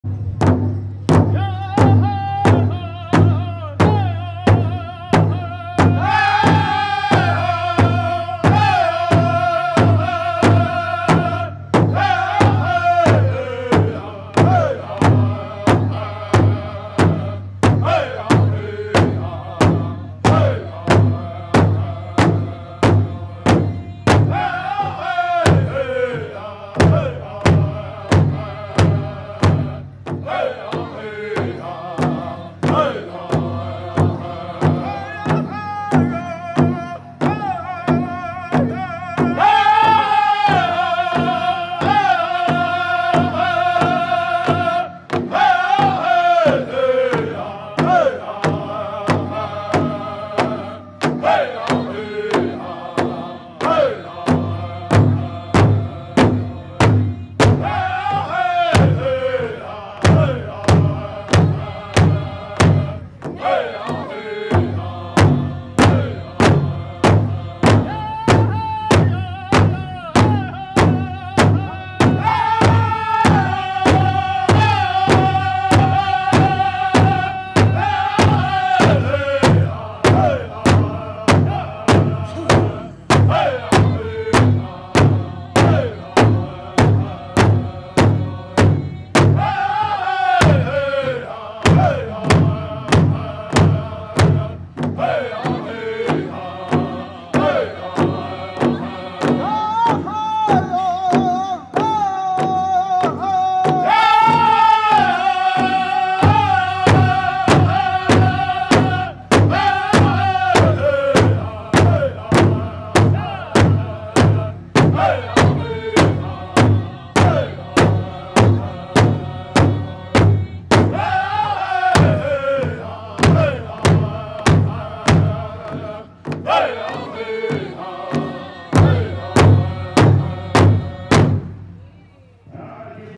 Buc 🦌 Wild (Sat. Afternoon Contest Song) @ Fort Hall Shoshone-Bannock Festival Pow-Wow 2019
Buc Wild Singers (Saturday Afternoon Contest Singers) singing during the Sho-Ban Festival Pow-Wow, Fort Hall, Idaho, August, 10, 2019.